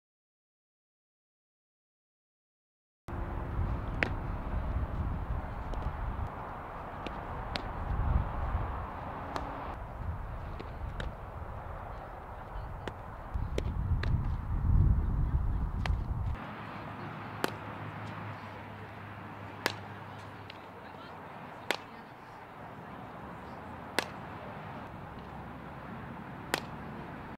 Hailey Clendennen - All-American Preview Showcase - Fastpitch Scouting Report
Hailey Clendennen's workout, filmed at the Colorado School of Mines, part of the All-American Preview Showcase by Fastpitch Scouting Report.